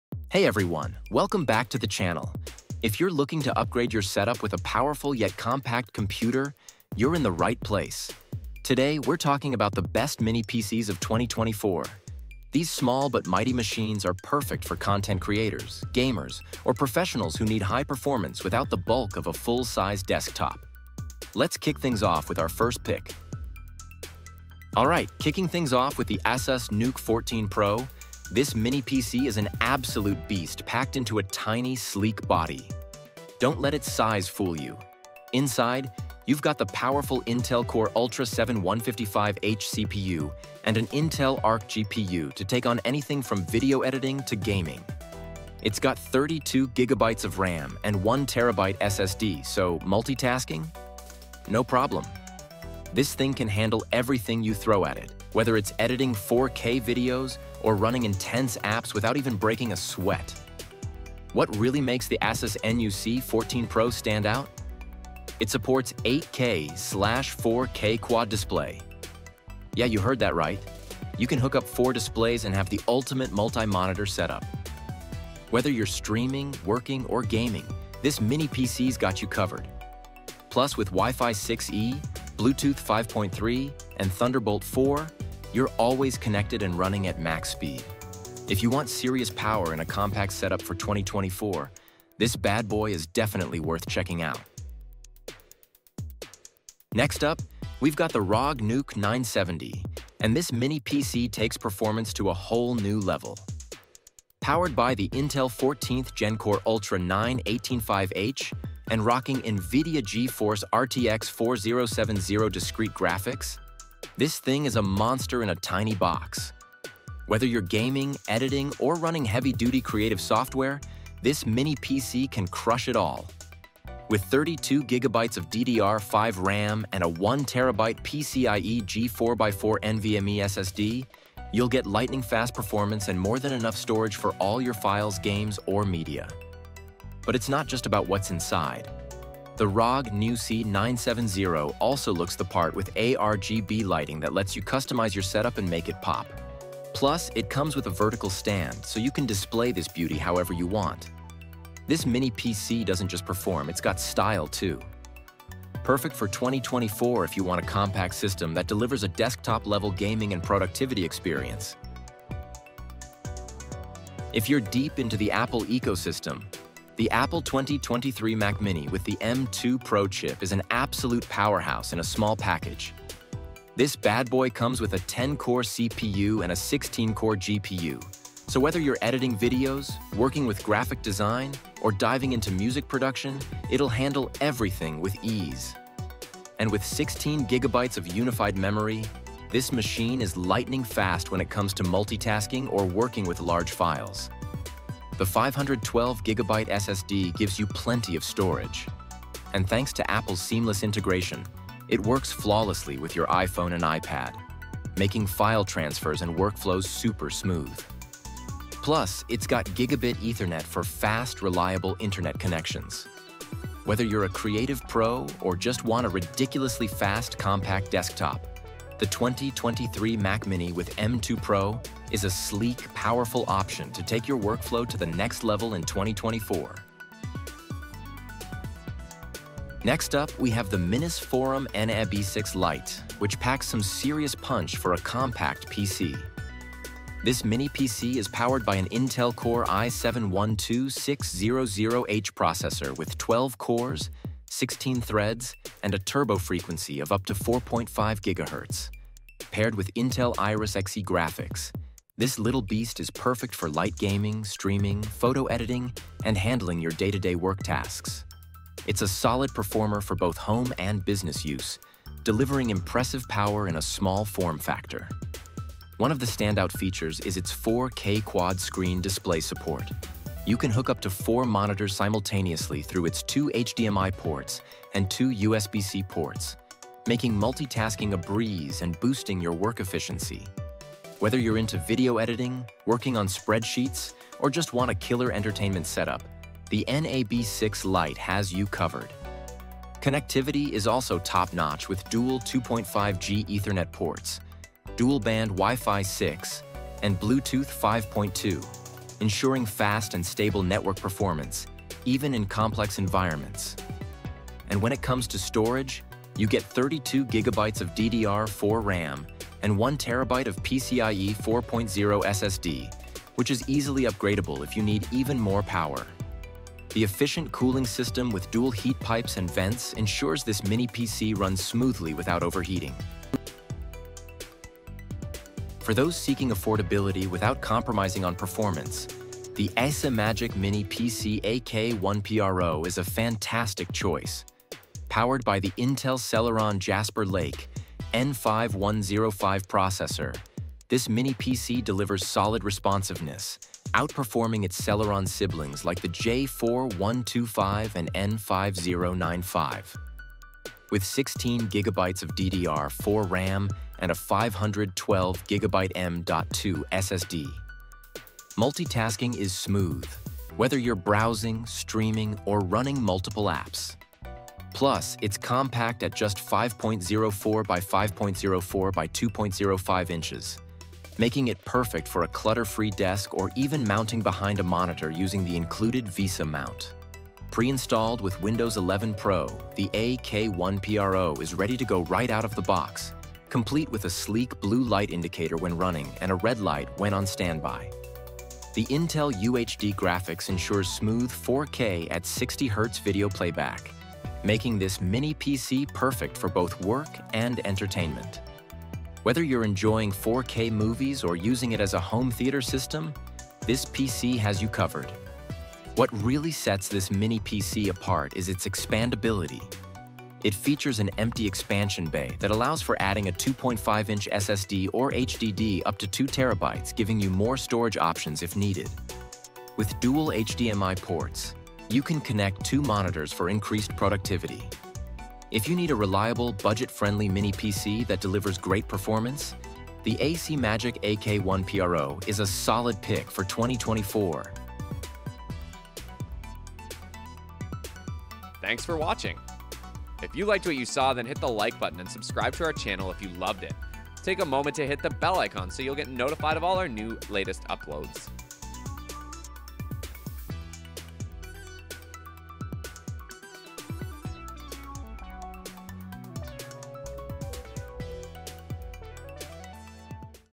The Ultimate Guide to the Best Mini PCs of 2024!
Check Out The Ultimate Guide to the Best Mini PCs Here!
► ASUS NUC 14 Pro
► ASUS ROG NUC 970
► Mac Mini M2 Pro
► MINISFORUM NAB6
► ACEMAGIC AK1 PRO

VIDEO GUIDE
00:00 The Ultimate Guide to the Best Mini PCs
00:24 Best Overall: ASUS NUC 14 Pro
01:36 Best for Gaming: ASUS ROG NUC 970
02:44 Best for Office Work: Mac Mini M2 Pro
03:49 Best for Entertainment: MINISFORUM NAB6
05:25 Best Budget: ACEMAGIC AK1 PRO

Hey everyone, welcome back to the channel! If you’re looking to upgrade your setup with a powerful yet compact computer, you’re in the right place. Today, we’re talking about the best mini PCs of 2024. These small but mighty machines are perfect for content creators, gamers, or professionals who need high performance without the bulk of a full-size desktop. Let’s kick things off with our first pick!

Best Overall: ASUS NUC 14 Pro
Alright, kicking things off with the ASUS NUC 14 Pro, this mini PC is an absolute beast packed into a tiny, sleek body. Don’t let its size fool you—inside, you’ve got the powerful Intel Core Ultra 7 155H CPU and an Intel Arc GPU to take on anything from video editing to gaming. It’s got 32GB of RAM and 1TB SSD, so multitasking? No problem. This thing can handle everything you throw at it, whether it's editing 4K videos or running intense apps without even breaking a sweat. What really makes the ASUS NUC 14 Pro stand out? It supports 8K/4K quad display! Yeah, you heard that right. You can hook up four displays and have the ultimate multi-monitor setup. Whether you’re streaming, working, or gaming, this mini PC’s got you covered. Plus, with Wi-Fi 6E, Bluetooth 5.3, and Thunderbolt 4, you’re always connected and running at max speed. If you want serious power in a compact setup for 2024, this bad boy is definitely worth checking out!

Best for Gaming: ASUS ROG NUC 970
Next up, we’ve got the ROG NUC 970, and this mini PC takes performance to a whole new level. Powered by the Intel 14th Gen Core Ultra 9 185H and rocking NVIDIA GeForce RTX 4070 discrete graphics, this thing is a monster in a tiny box! Whether you’re gaming, editing, or running heavy-duty creative software, this mini PC can crush it all. With 32GB of DDR5 RAM and a 1TB PCIe G4x4 NVMe SSD, you’ll get lightning-fast performance and more than enough storage for all your files, games, or media. But it’s not just about what’s inside. The ROG NUC 970 also looks the part with ARGB lighting that lets you customize your setup and make it pop. Plus, it comes with a vertical stand, so you can display this beauty however you want. This mini PC doesn’t just perform, it’s got style too. Perfect for 2024 if you want a compact system that delivers a desktop-level gaming and productivity experience!

Best for Office Work: Mac Mini M2 Pro
If you’re deep into the Apple ecosystem, the Apple 2023 Mac Mini with the M2 Pro chip is an absolute powerhouse in a small package. This bad boy comes with a 10-core CPU and a 16-core GPU, so whether you’re editing videos, working with graphic design, or diving into music production, it’ll handle everything with ease. And with 16GB of unified memory, this machine is lightning fast when it comes to multitasking or working with large files. The 512GB SSD gives you plenty of storage, and thanks to Apple’s seamless integration, it works flawlessly with your iPhone and iPad—making file transfers and workflows super smooth. Plus, it’s got Gigabit Ethernet for fast, reliable internet connections. Whether you're a creative pro or just want a ridiculously fast, compact desktop, the 2023 Mac Mini with M2 Pro is a sleek, powerful option to take your workflow to the next level in 2024!